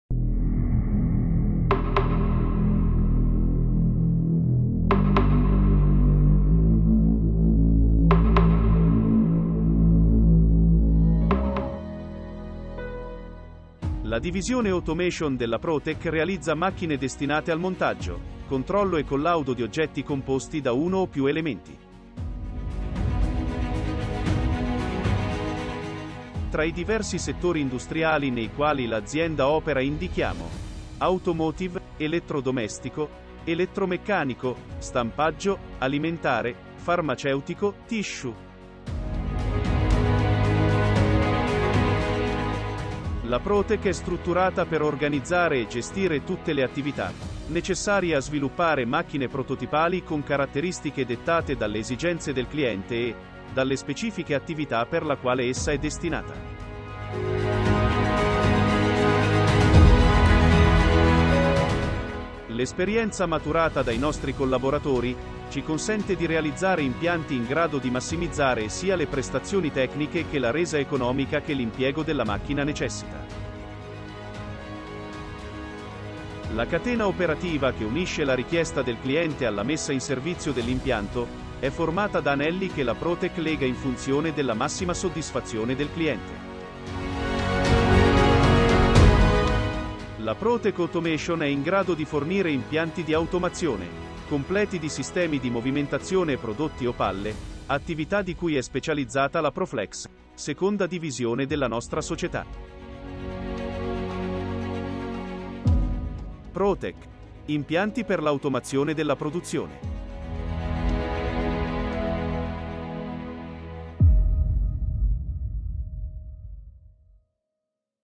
PROTEK Automation | Presentazione generale
ProTek progetta, realizza e installa impianti di automazione e movimentazione industriale (convogliatori), con esperienza e referenze soprattutto nel settore alimentare, cosmetico, farmaceutico, automotive e tissue.
La ProTek ha l’esperienza e l’organizzazione per fornire macchine e impianti speciali per l’automazione industriale e in particolar modo per:
- Assemblaggio
- Marcatura
- Controlli estetici (sistemi di visione)
- Controlli dimensionali (a contatto e non)
- Controlli strutturali dei metalli (correnti indotte)